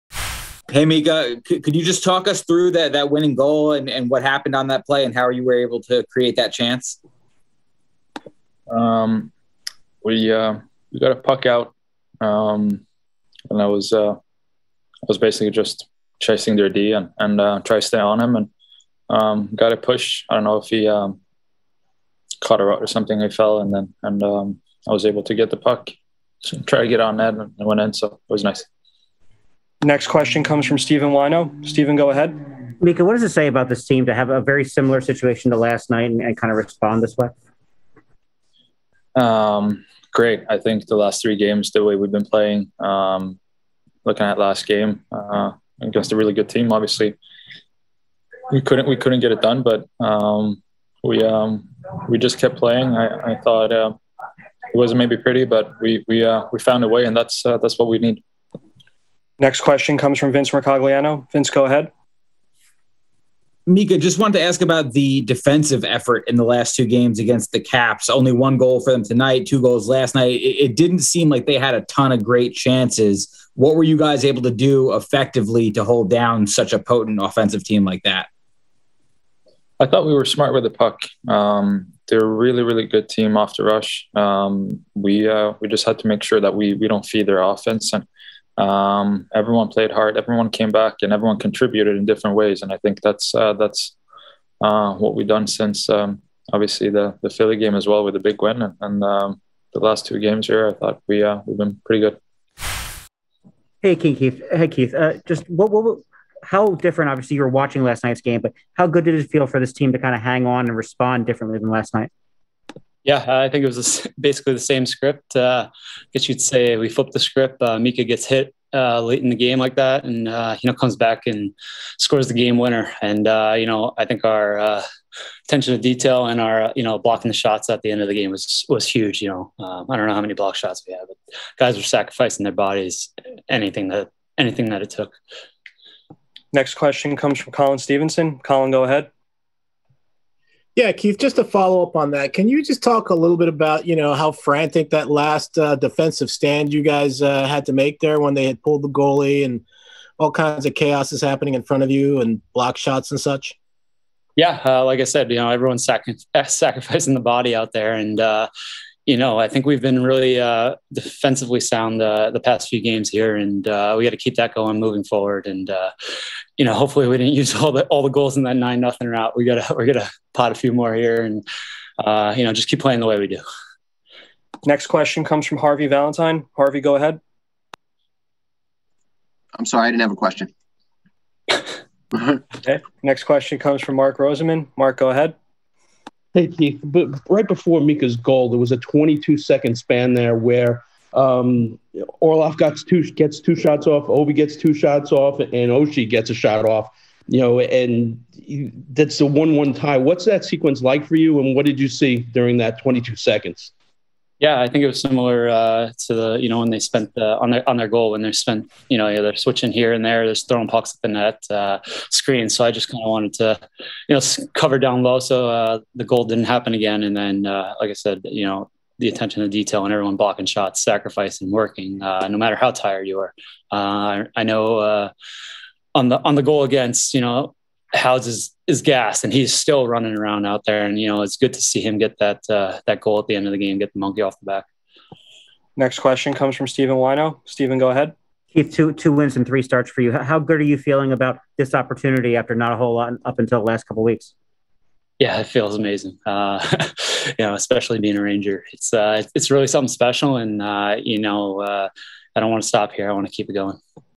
Zibanejad & Kinkaid React to Win Over Caps and Mika's HUGE Goal | New York Rangers
Mika Zibanejad scored the winning goal and Keith Kinkaid had 28 saves in a strong win vs the Washington Capitals.